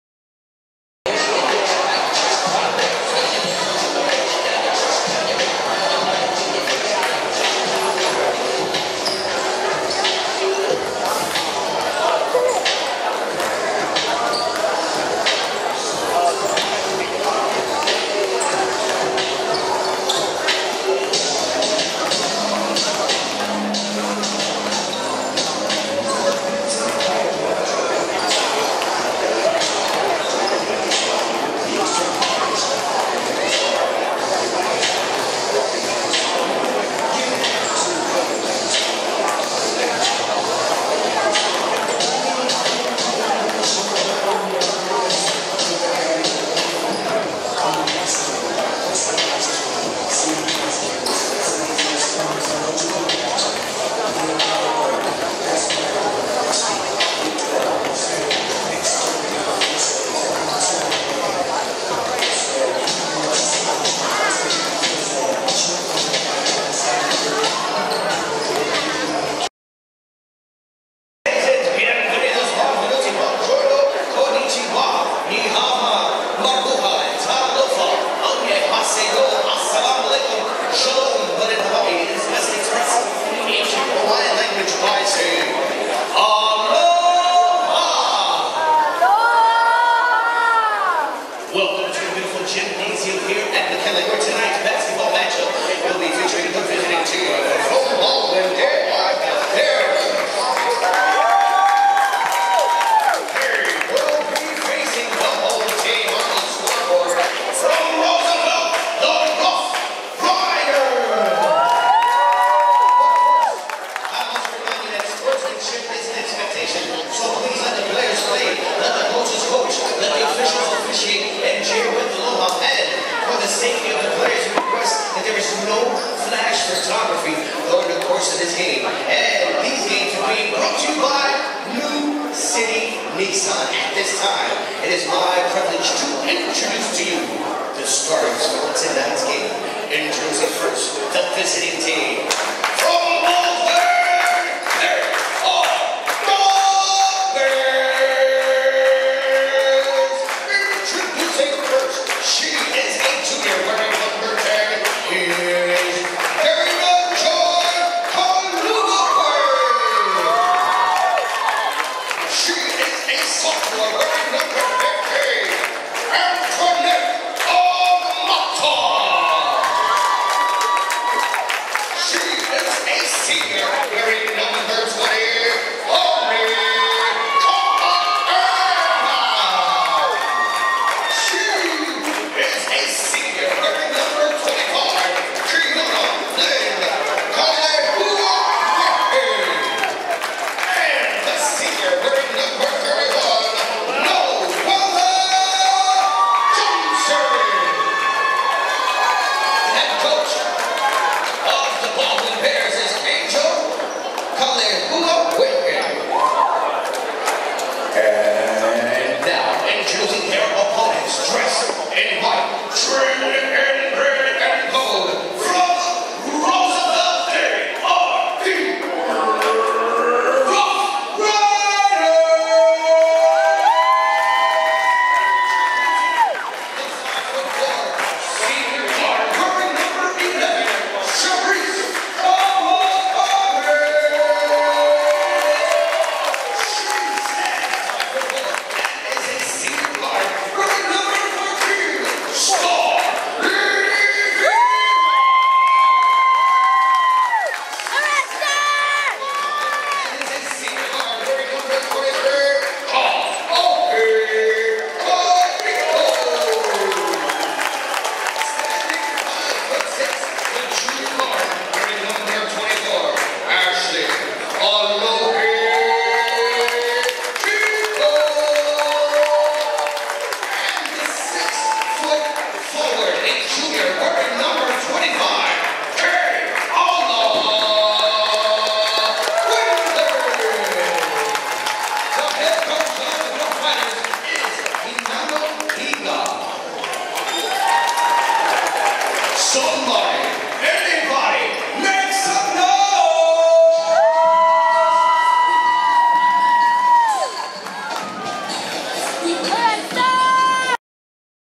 2015 State Girls Basketball - Roosevelt vs Baldwin - Introductions 2-11-15
click2ed videos

Roosevelt Rough Riders vs Baldwin Bears
Hawaii State Girls Basketball Championships
February 11, 2015 - McKinley High School Gym